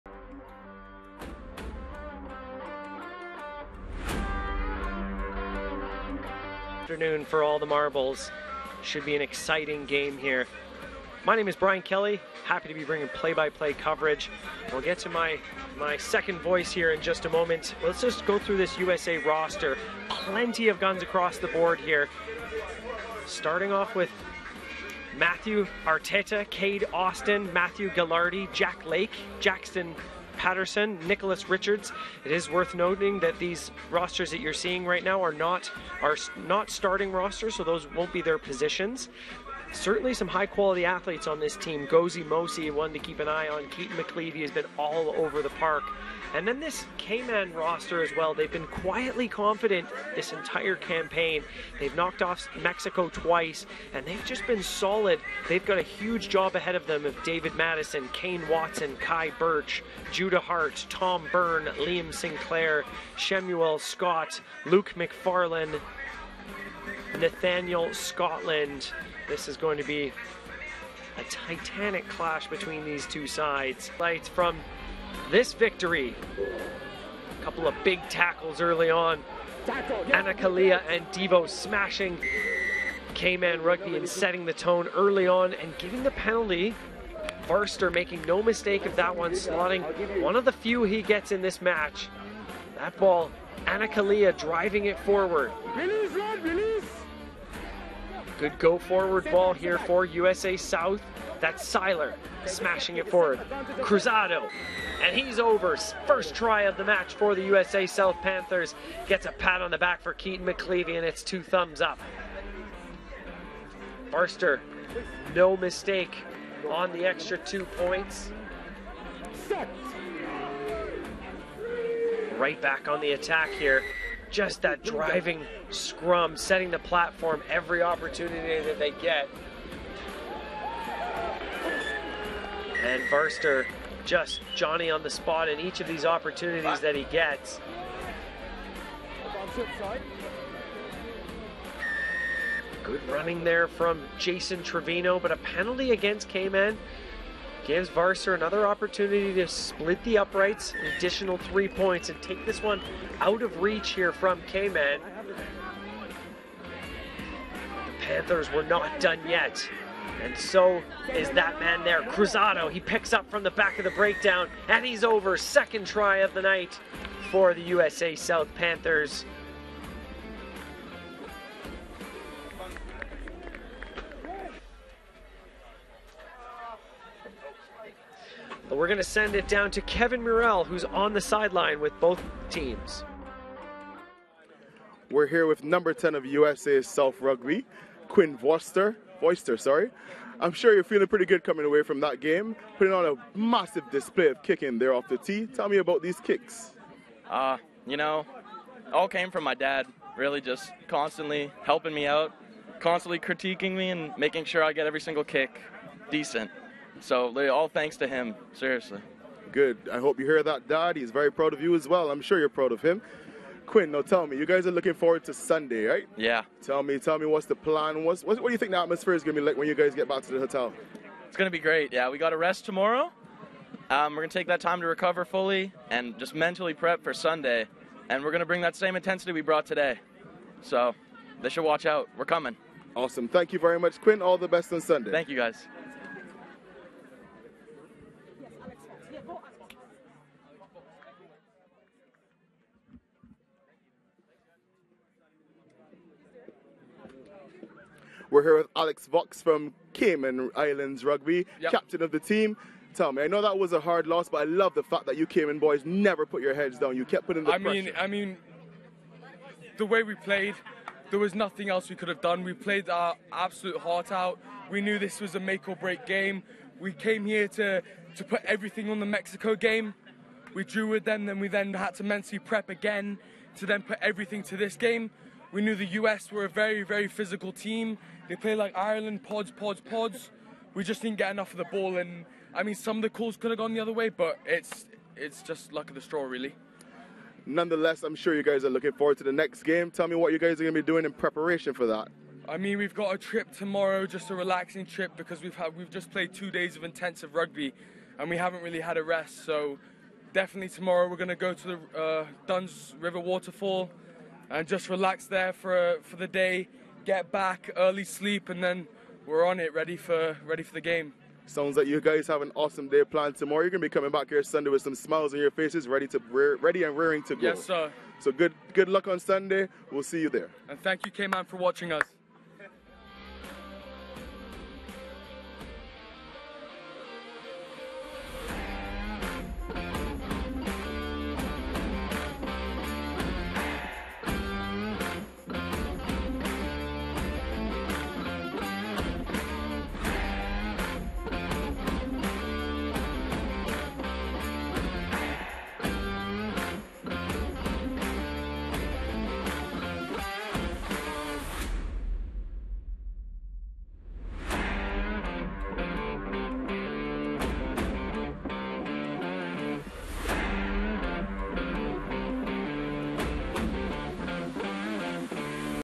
Men’s U19: USA South blank Cayman Islands 29-0 in Rugby Americas North Tournament | SportsMax
Watch the full Rugby Americas North Tournament highlights between USA South and Cayman Islands.